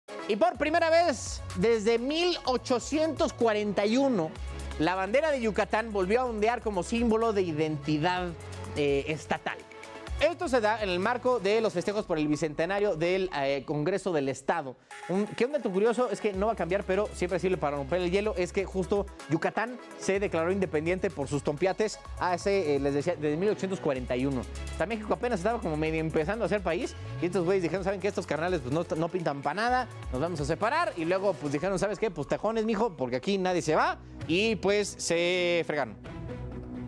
Después de 182 años, izan por primera vez la bandera de Yucatán
Luego de 182 años, se llevó a cabo la ceremonia cívica del izamiento de la bandera de Yucatán, que vuelve a ondear como símbolo de la identidad del pueblo yucateco, evento presidido por el gobernador Mauricio Vila Dosal.

Por primera vez, desde 1841, dicho lábaro patrio volvió a ondear, ahora a la par de la bandera de México, como acto para refrendar el compromiso del Estado con el Pacto Federal y fomentar el patrimonio cultural, historia e identidad del pueblo yucateco.